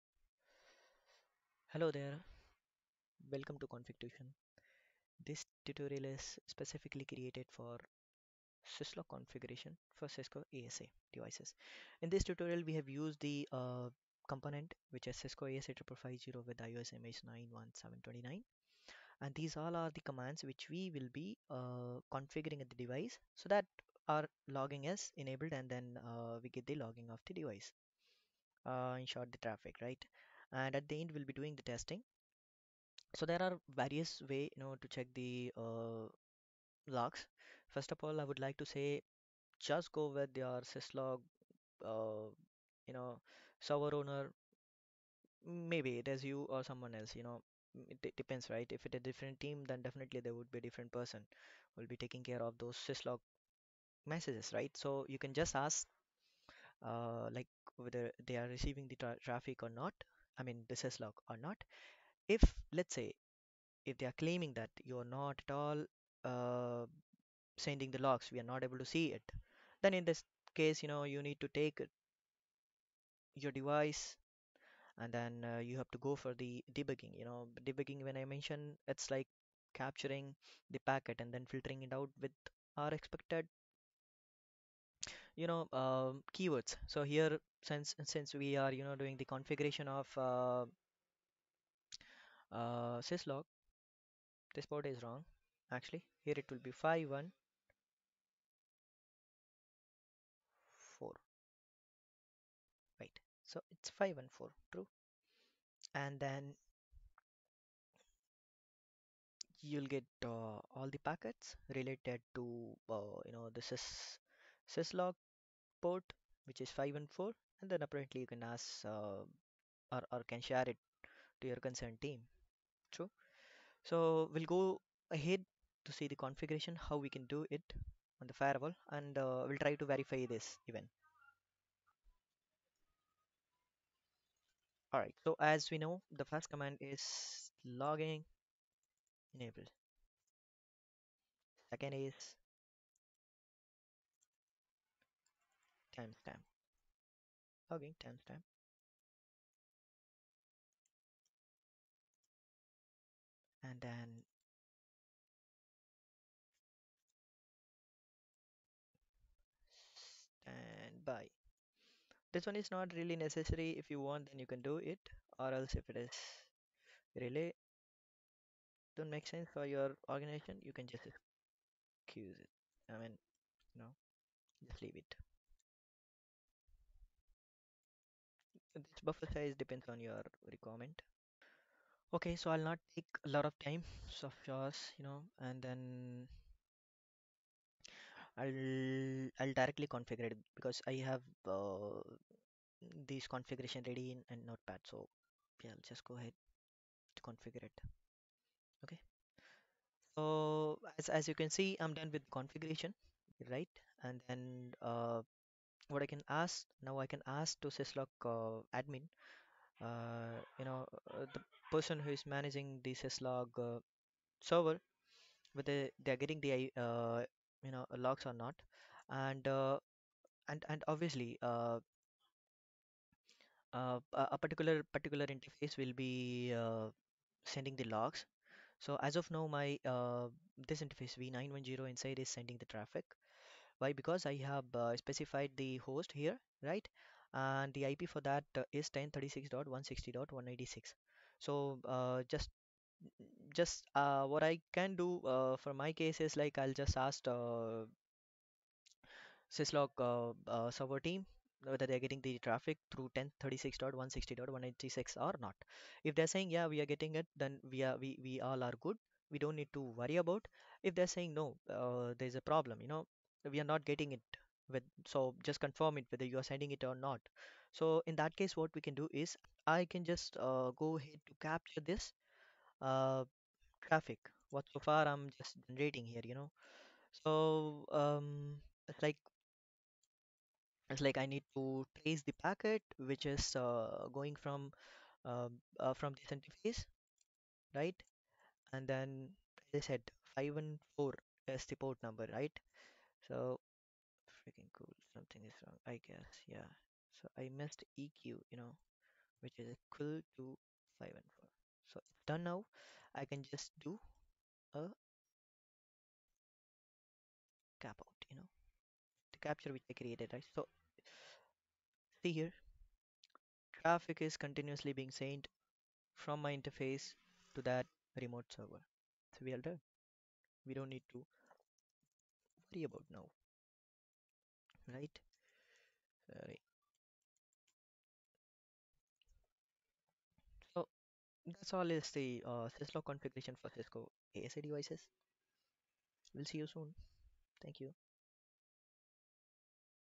Cisco ASA Syslog Configuration | Cisco ASA 55xx | Cisco ASA Firewall | Easy Configuration Guide
this tutorial is created for Syslog configuration for the Cisco 55xx series firewall.

Below is the configuration which can be used for applying directly to the appliances.

1. Configuration command :

logging enable
logging timestamp
logging standby
logging buffer-size 100000
logging buffered debugging
logging trap informational
logging history warnings
logging asdm debugging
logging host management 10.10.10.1
logging message 106101 level informational

2. Debug command :

capture capout interface management trace match udp any any eq 514

--- that's it. if traffic is passed from the firewall but not received at Syslog sever end you can check for the in-between devices if any acl/firewalling is being done.